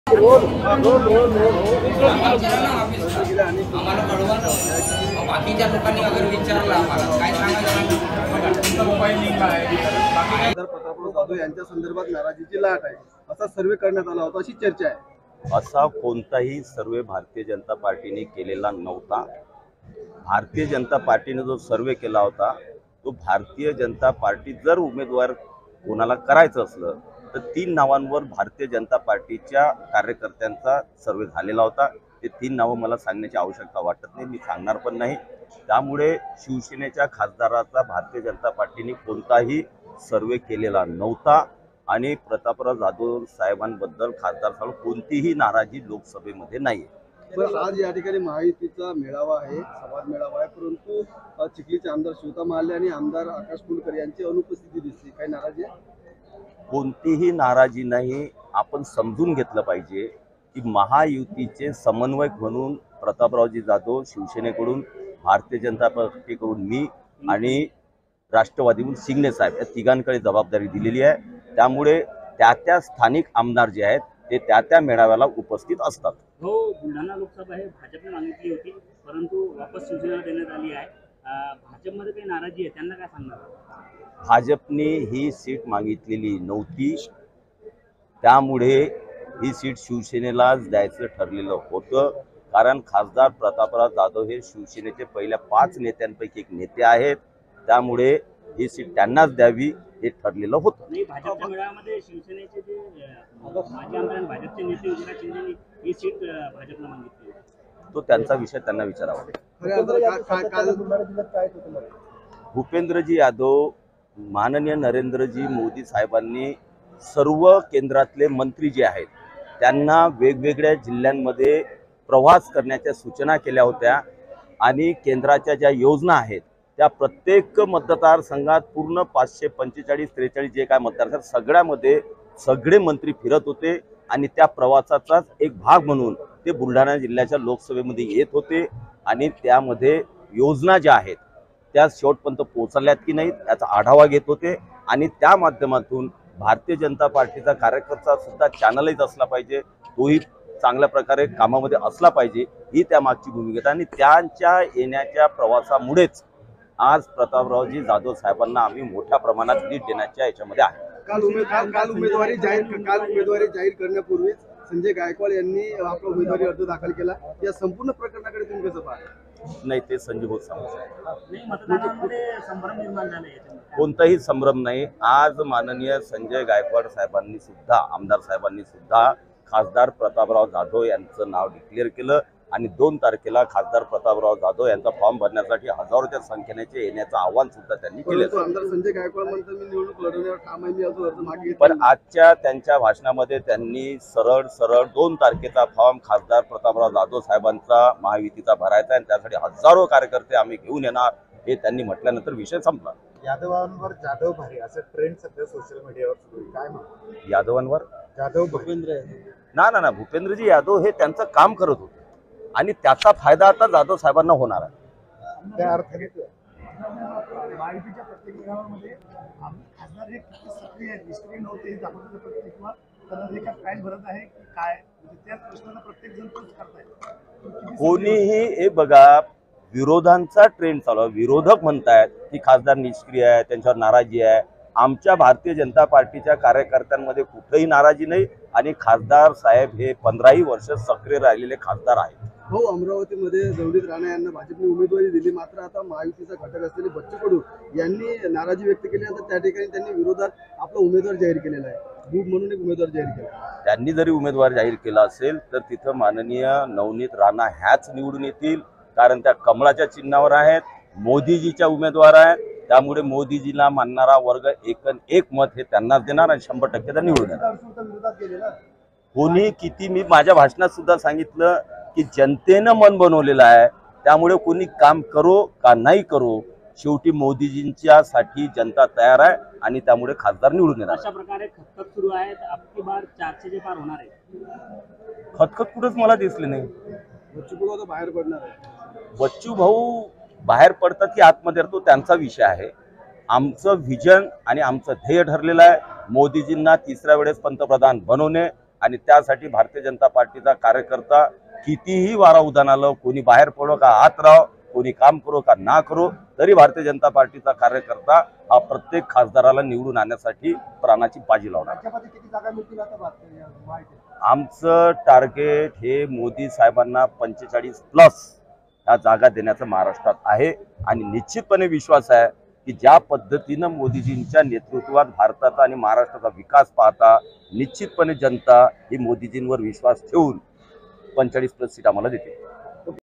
BULDHANA | आमदारांमध्ये नाराजी असल्याचा कोणताही सर्व्हे भारतीय जनता पार्टी ने केला नव्हता .संजय कुटे
-आमदारांमध्ये कोणतीही नाराजी नाही स्थानिक आमदार त्या-त्या मतदार संघाच्या मेळाव्यात उपस्थित राहतात .
- नरेंद्र मोदी, भुपेंद्र यादव यांनी केंद्रातील सर्व मंत्र्यांना सुचना केल्या आहेत सर्व जिल्ह्यांमध्ये तसेच सर्व मतदार संघात फिरुन केंद्र सरकारच्या सर्व योजनांची सविस्तर माहीती मतदारांपर्यंत पोहचवा अशा प्रकारच्या प्रवास  सूचना केल्या होत्या. त्याच माध्यामातुन भाजपा नेते बुलढाण्यात येत होते केंद्राच्या योजना देशातील जनतेपर्यंत पोचल्या की नाही याचा आढावा घेत होते.
- आमदार संजय गायकवाड यांचा ऊमेदवारीचा विषय संपला..
 भूपेंद्र यादव हे त्यांचे काम करत होते त्याचा फायदा आता जाधव साहेबांना होणार आहे..
भारतीय जनता पार्टीच्या कार्यकर्त्यांमध्ये कुठलीही नाराजी नाही हे जाधव पंधरा वर्ष सक्रिय राहिलेले खासदार आहेत

BYTE - डॉ संजय कुटे आमदार तथा प्रदेश उपाध्यक्ष भारतीय जनता पार्टी..